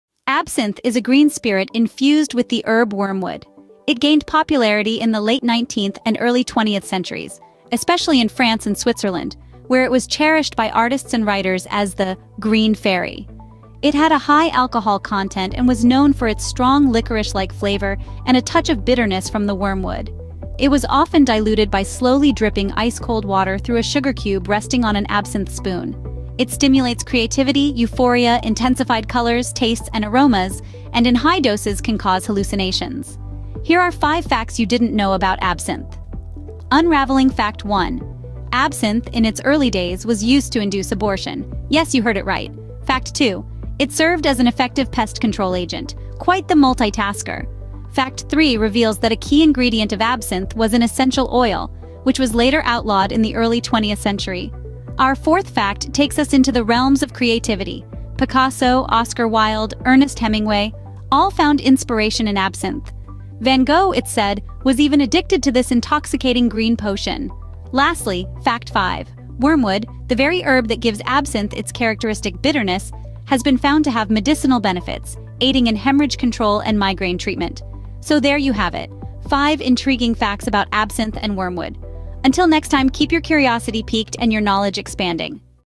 ✨ 5 UNBELIEVABLE FACTS about Absinthe & Wormwood! 🥂
🥂 Absinthe is a legendary green spirit infused with the herb wormwood, among other botanicals. This iconic drink is created through a meticulous process involving maceration and distillation, typically including green anise, fennel, and various herbs. It gained immense popularity in the late 19th and early 20th centuries, especially in France and Switzerland, where it was cherished by artists and writers as the "Green Fairy."

This video explores 5 unbelievable facts about Absinthe & Wormwood! 🌿